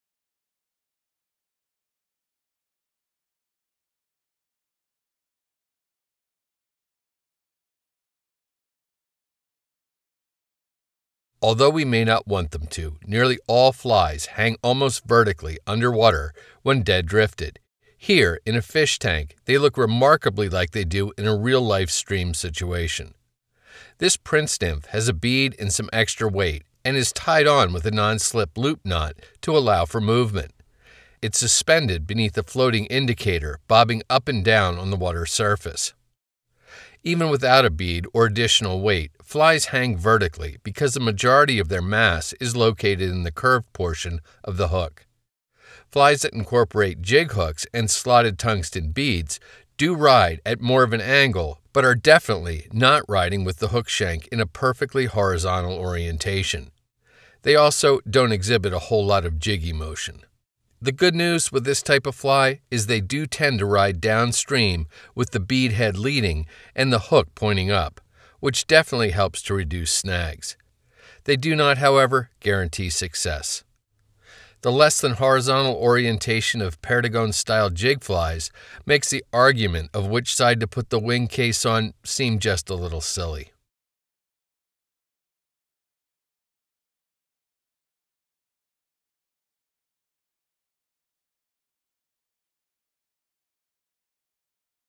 Underwater Fly Orientation
Video shows how different types of flies orient underwater.